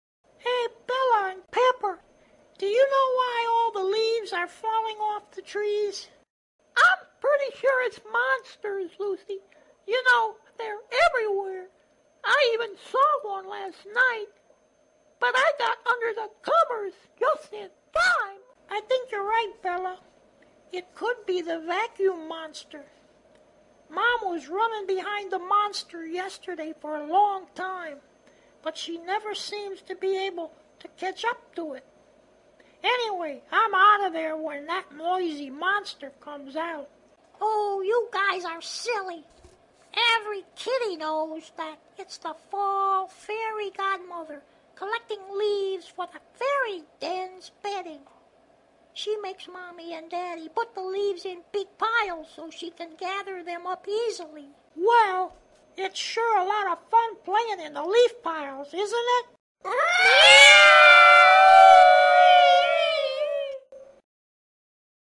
Lucy Bella Pepper Season Change.wmv
Talking dogs and cat ponder why the leaves are falling